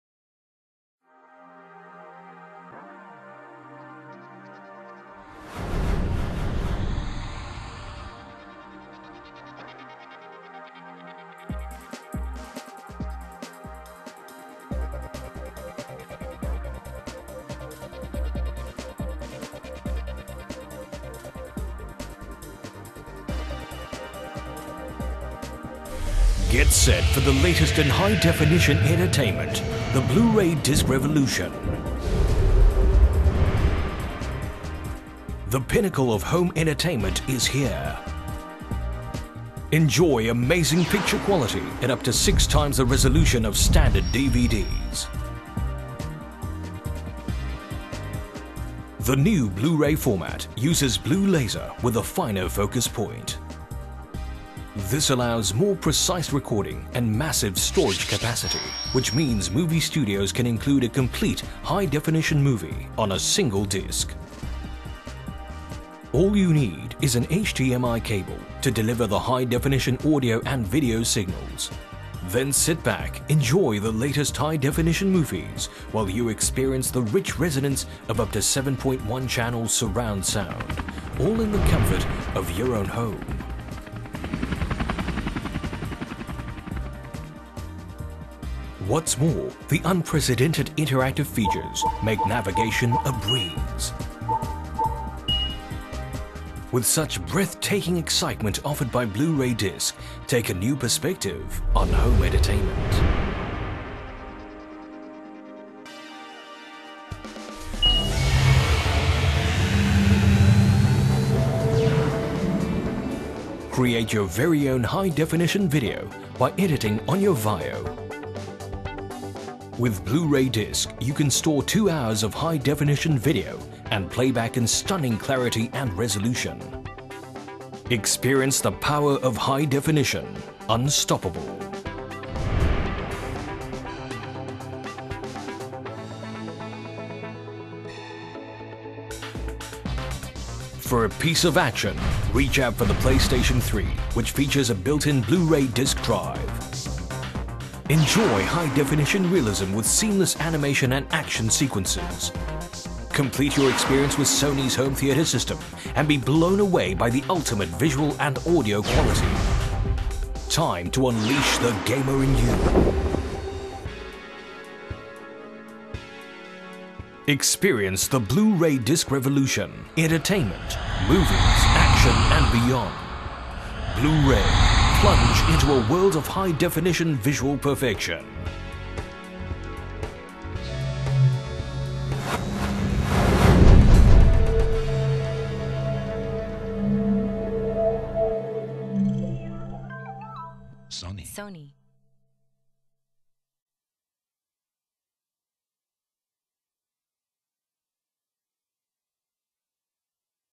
Blu-ray Disc Demo Video [2007]
Full HD video with 7.1 ch sound (on master) to explain the benefits of Blu-ray Disc.

For Sony Electronics Asia Pacific.